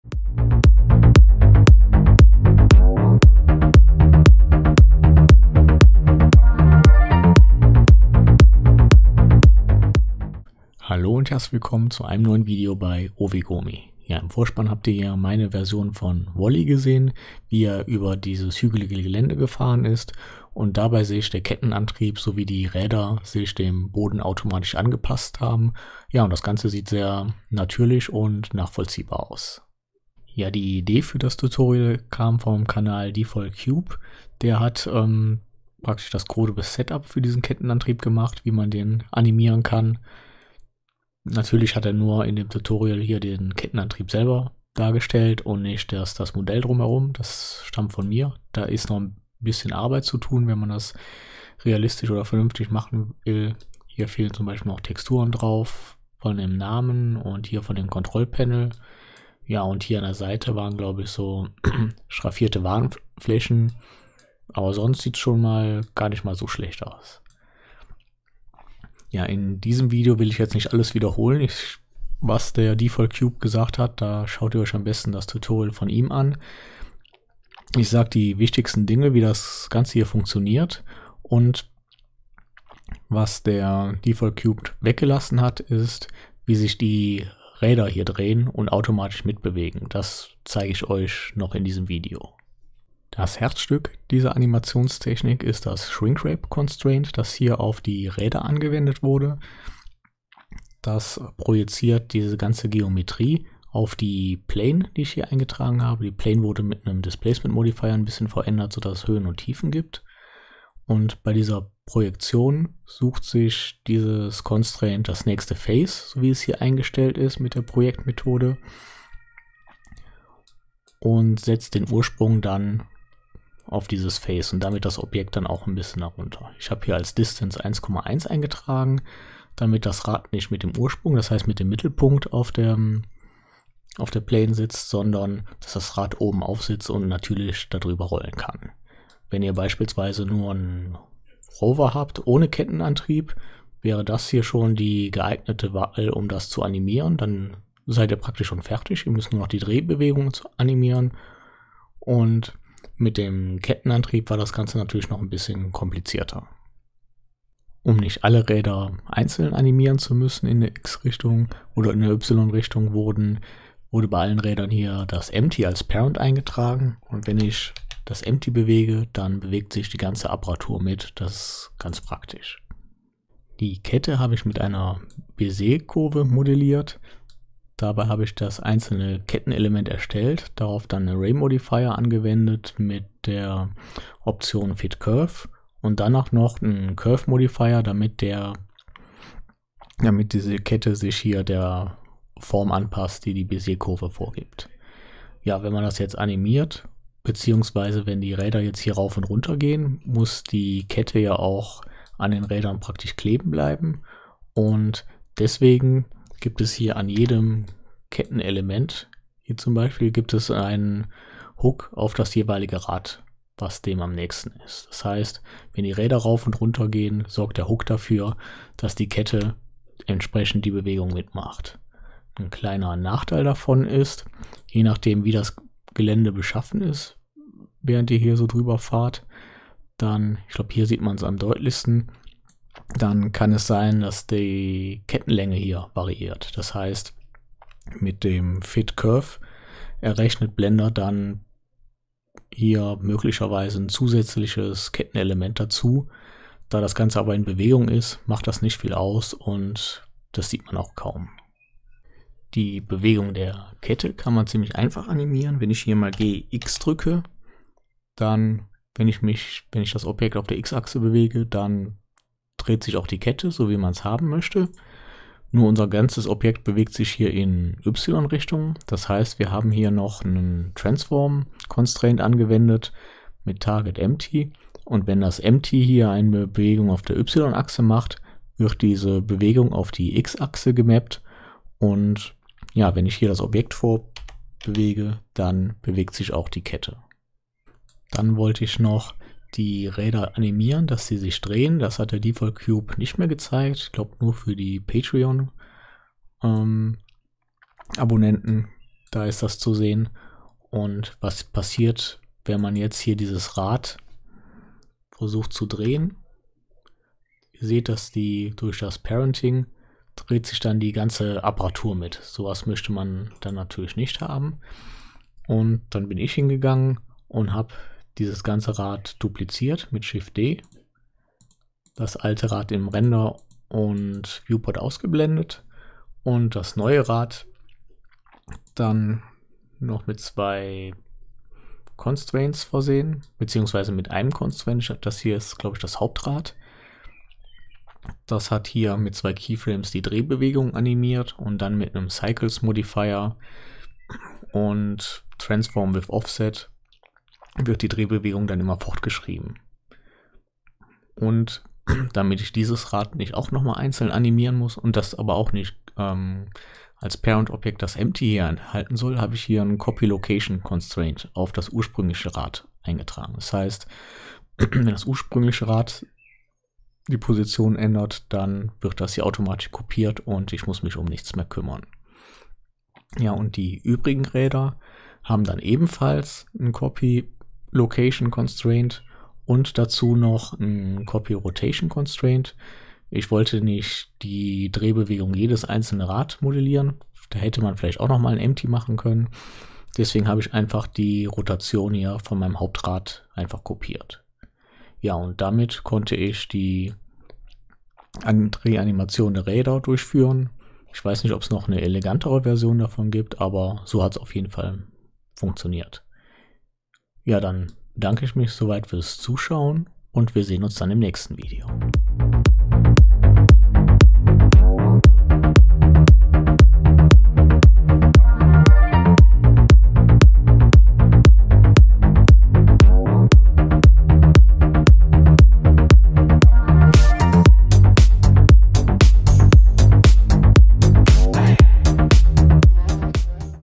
Animation Kettenantrieb
In diesem Video zeige ich euch die wichtigsten Punkte aus dem Video von Default Cube zur Animation eines Kettenantriebs.

Im letzten Abschnitt zeige ich euch  eine Methode wie ihr die Räder drehen lassen könnt. Dies wurde im Video von Default Cube nur den Patreons gezeigt.

Inhalt:
0:00 Intro
0:30 Default Cube
1:32 Shrinkwrap Constraint
2:37 Parenting
2:53 Modellierung  und Animation Kette
4:47 Drehen Räder